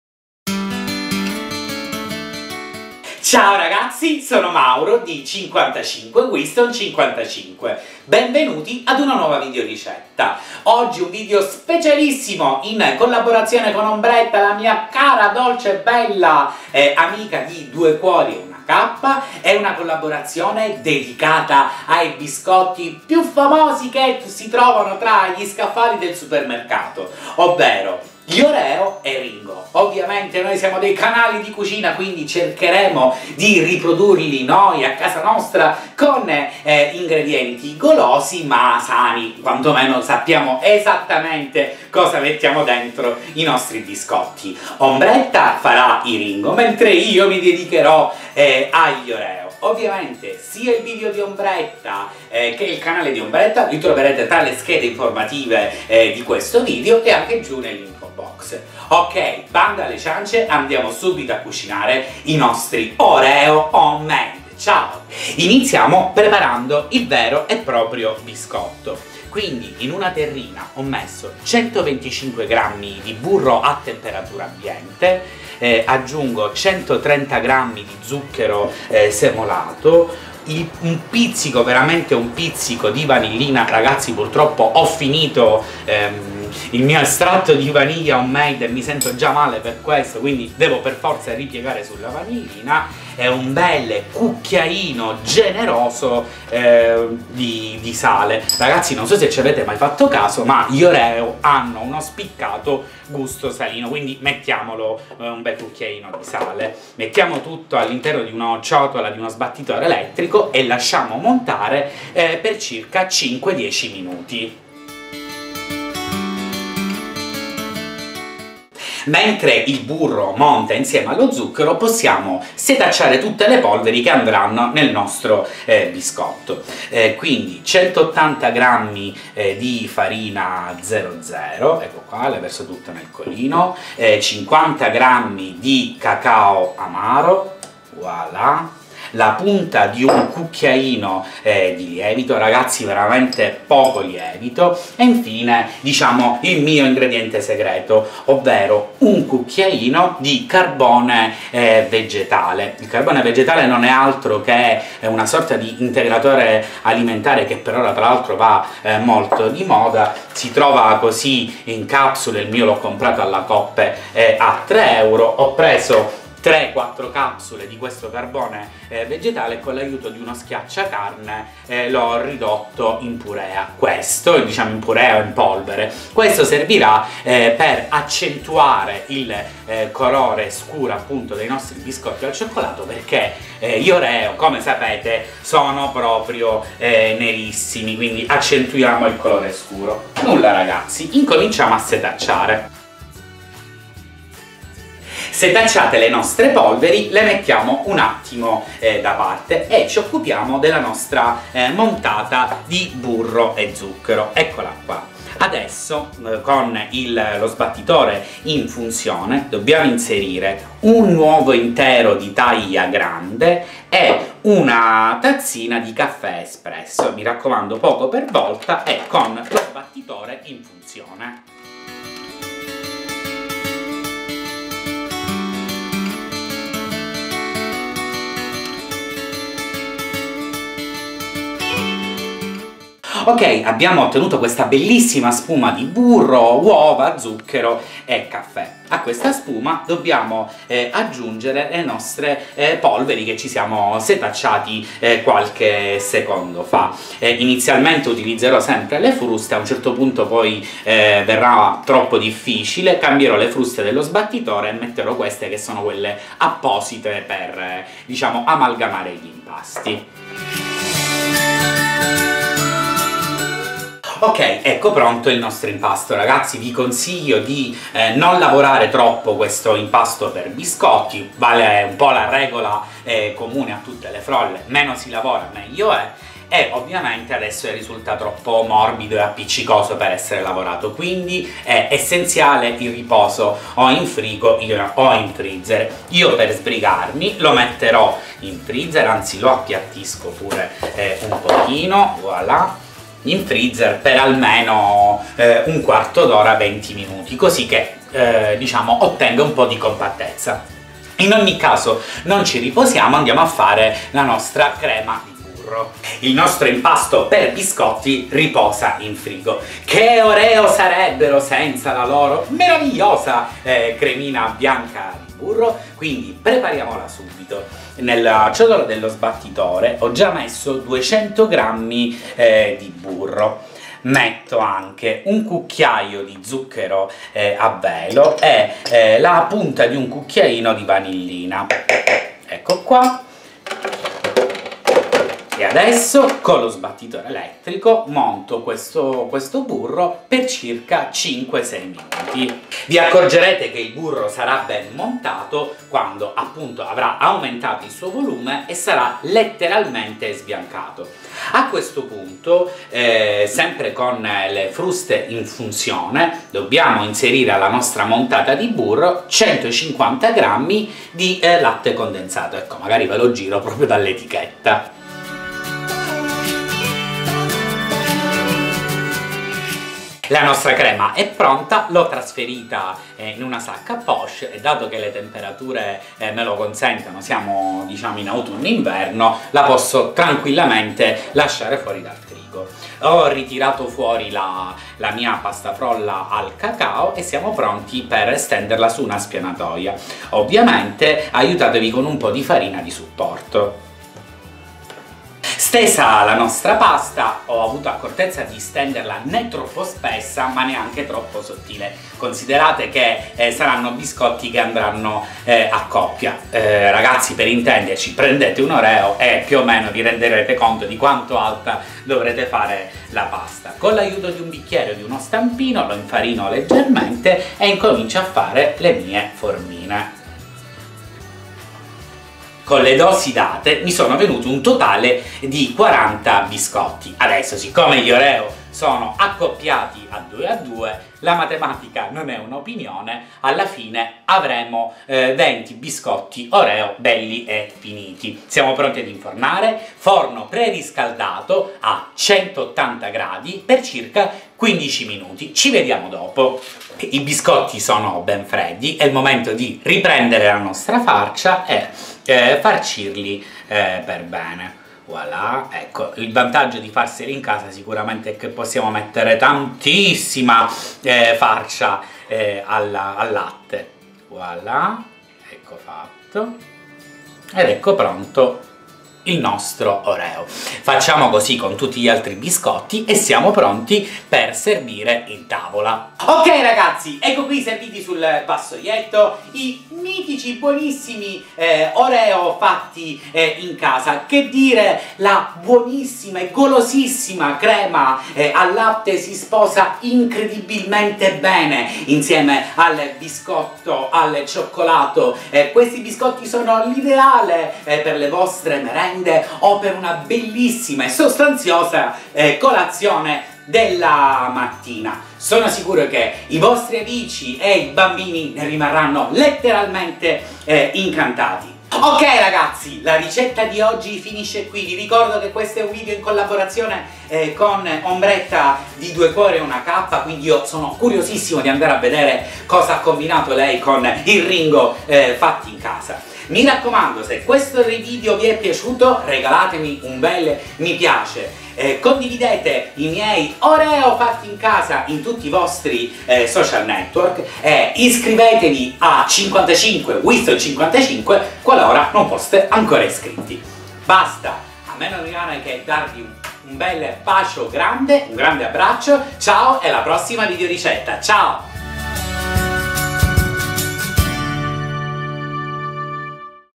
Biscotti Oreo Fatti in Casa | Ricetta Facile e Golosa | 55Winston55
Ciao ragazzi, oggi prepariamo insieme i Biscotti Oreo Fatti in casa!
Sono sempre stato un fan di questi biscotti e quindi ho messo a punto una ricetta che utilizza ingredienti sani e alla portata di tutti! State pur certi che nella mia versione non c'è traccia di olio di palma, coloranti o altri ingredienti strani! Sono dei biscotti ideali per una colazione o una merenda ricca e golosa! Chi non ama una fragante pasta frolla al cioccolato che racchiude un morbidissimo e scioglievole ripieno di crema al latte? Bambini, ragazzi, amici e parenti...insomma proprio tutti, rimarranno conquistati da questi Oreo fatti in casa! Provare per credere!
Un abbraccio e buona Visione
55Winston55

❤NUOVI VIDEO ogni Mercoledì & Venerdì ore 14.00 e Domenica ore  9:00

***INGREDIENTI***
...per la frolla:
180 gr. farina "00"
125 gr. burro
130 gr. zucchero
50 gr. cacao amaro
1 uovo intero grande
1 tazzina di caffè
1 pzc. di vanillina
1 cucchiaino scarso di sale
1 pzc. lievito per dolci
1 cucchiaino di carbone vegetale

...per la crema al latte:
200 gr. burro
150 gr. latte condensato
1 cucchiaio di zucchero a velo
1 pzc. di vanillina